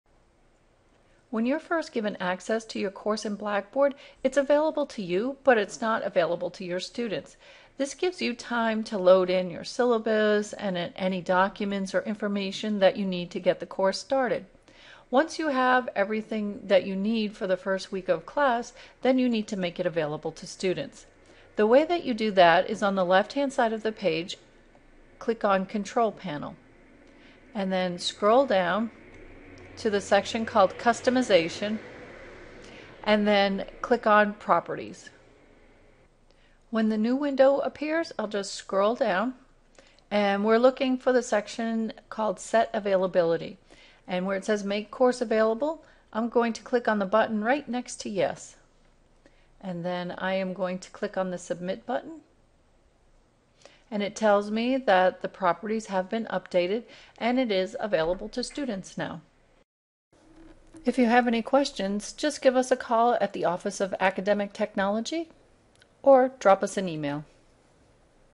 Faculty: Make Course Available in Blackboard
How to make a course in Blackboard available to students.